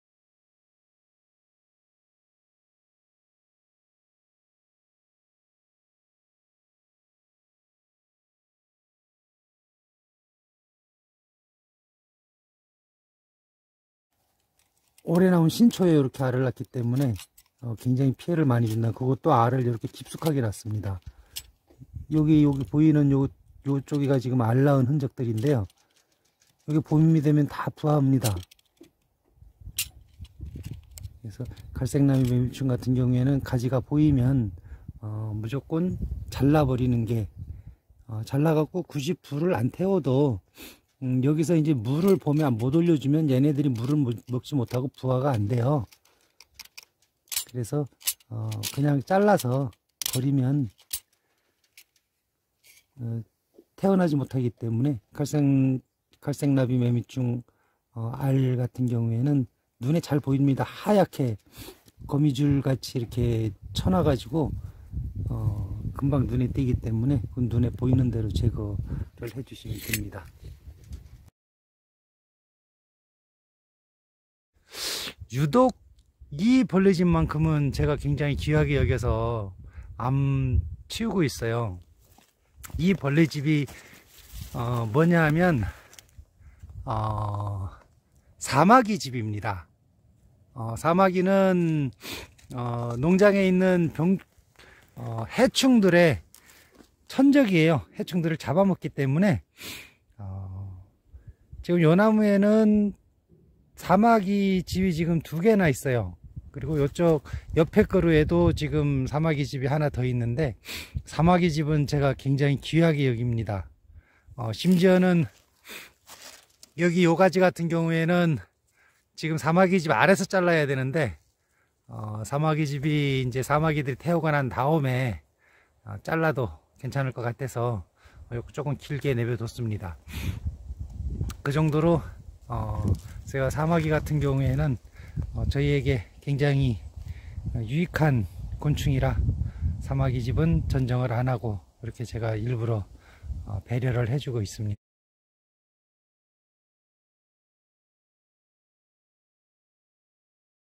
3월 해충알집 하나 제거 와 익충알집 보호 친환경 농사의 필수
산초나무를 친환경 초생재배하는 화전민산초농장에서는
3월 이면 해충알 제거에 집중합니다.
몇백마리 잡는 효과가 있습니다.
그러나 익충알집은 보호 해야겠지요